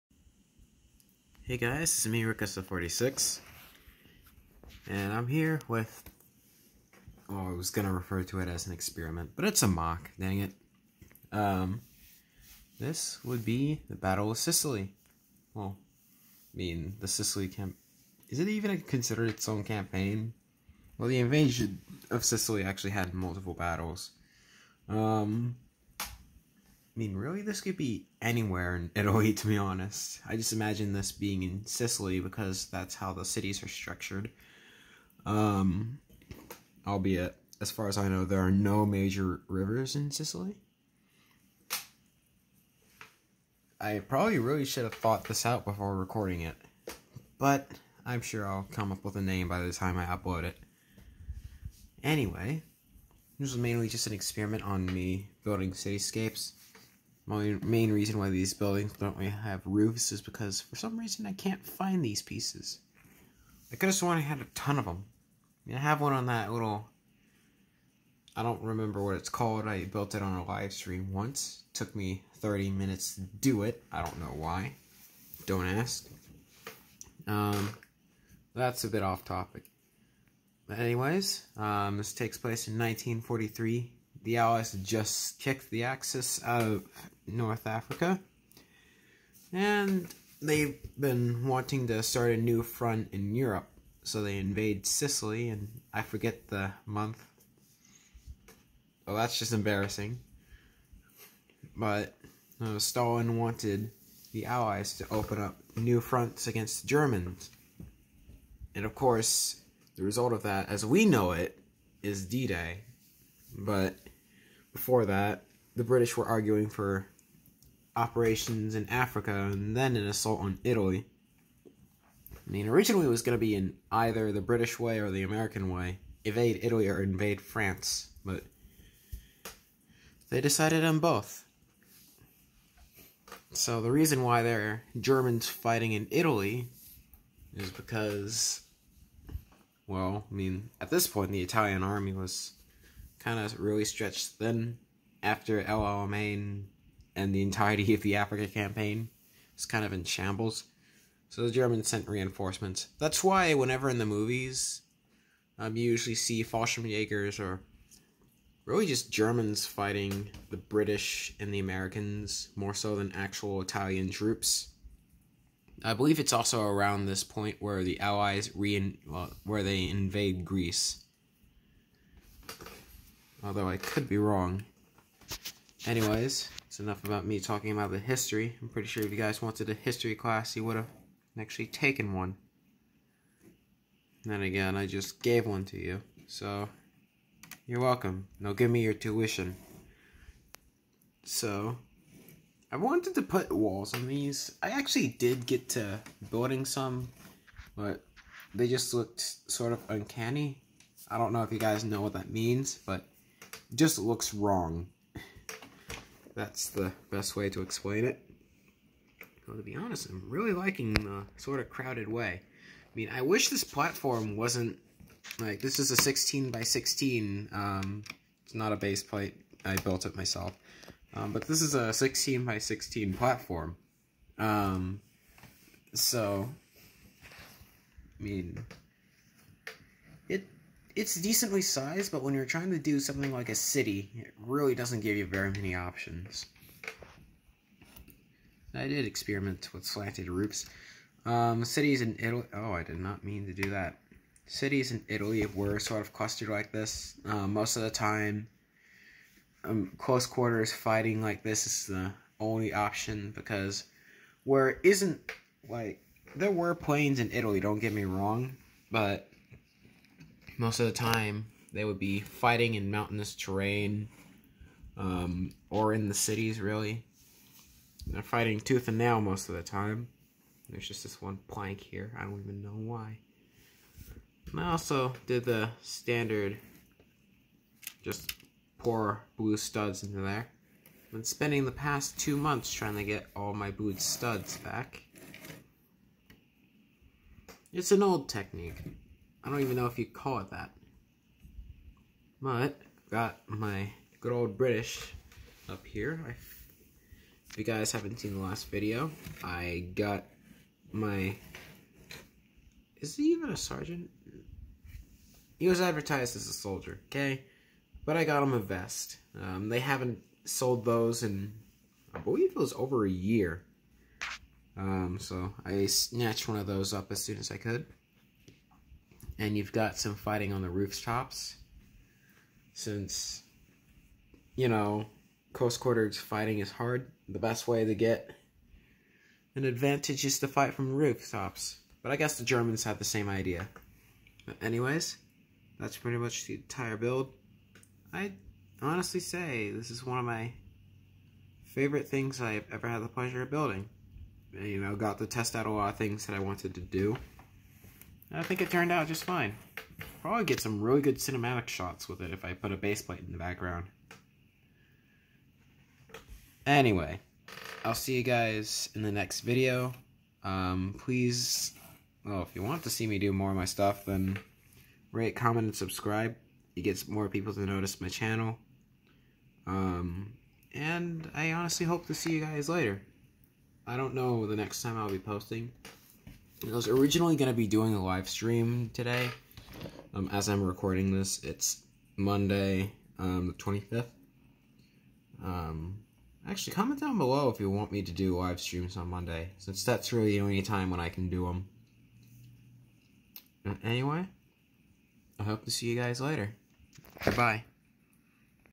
Operation Husky MOC
If you didn't already know, Operation Husky is the codename for the Allied invasion of Sicily in 1943. Famous for it's close quarters city combat.

I made this as an experiment in how I could do a close quarters combat scene. I think it turned out pretty good.